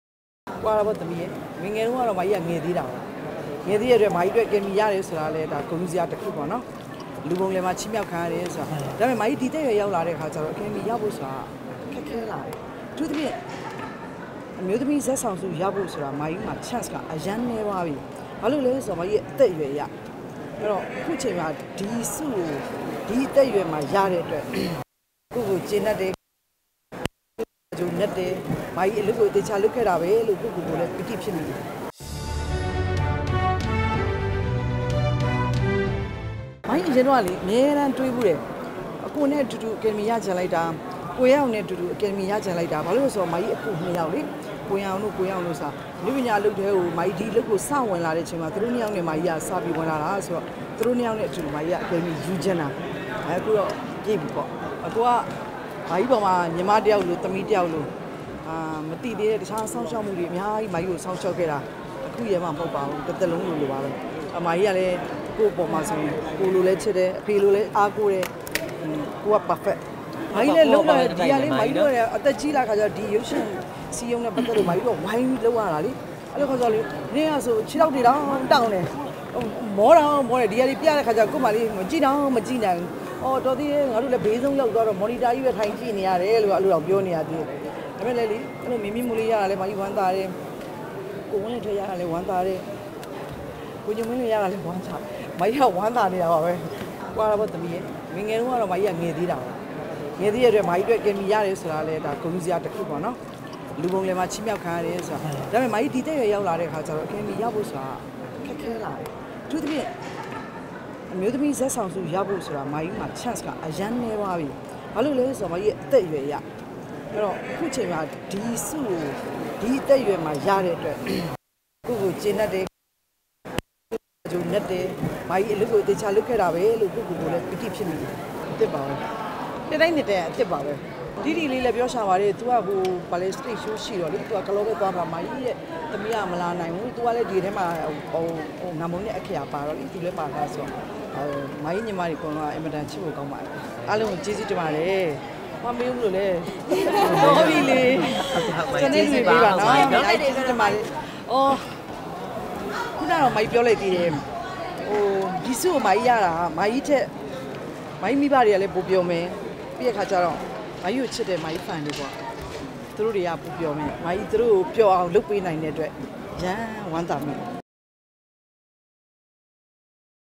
ဒီအသက္အရြယ္မွာ အမျိုးသမီးဇာတ်ဆောင်ဆု ရဖို့ဆိုတာ မကြီးအတြက္ Chance အရမ်းနည်းသွားပြီ
မကြီးရဲ့ အသက်အရွယ်အရ ခုချိန်မှာ ဒီဆုကို ဒီအသက်အရွယ်မှာ ရတဲ့အတွက် မကြီးကျေနပ်တယ် အလုပ်ကိုသေချာလုပ်ခဲ့တာပဲလို့ ပီတိဖြစ်မိတယ်